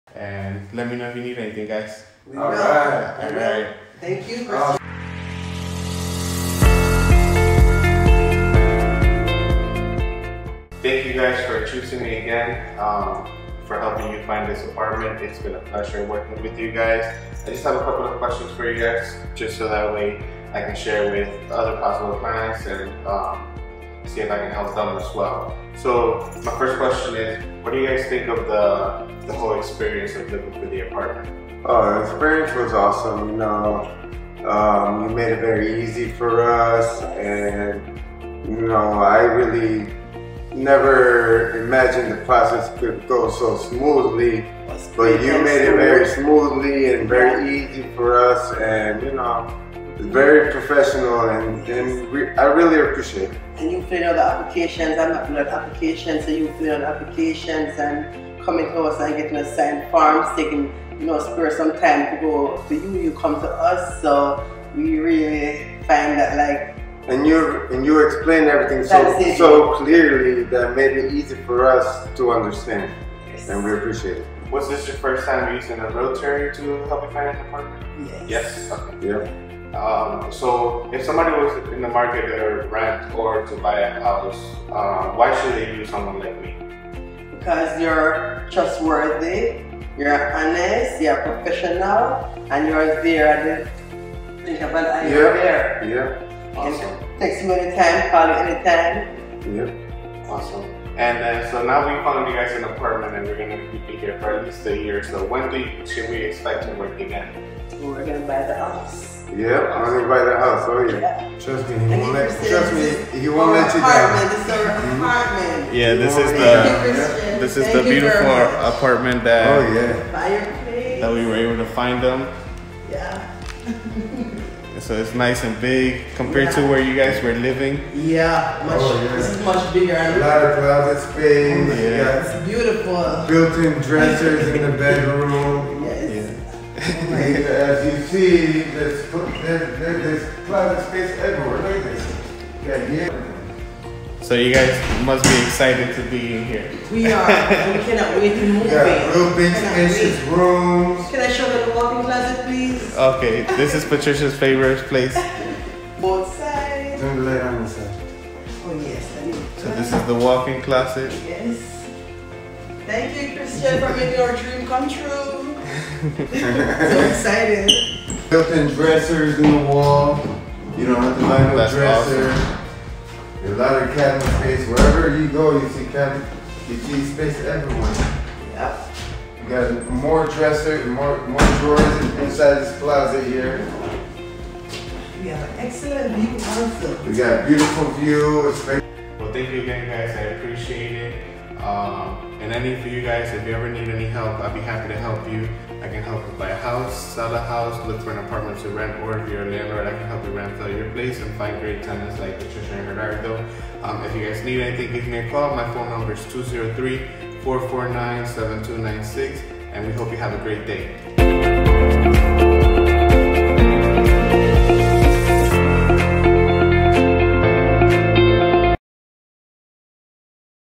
Gerardo & Patricia’s Real Estate Testimonial
Our clients Gerardo and Patricia are super excited to have found the apartment of their dreams!  They were a pleasure to work with and I look forward to helping them in the near future.  We hope to help you achieve your real estate goals.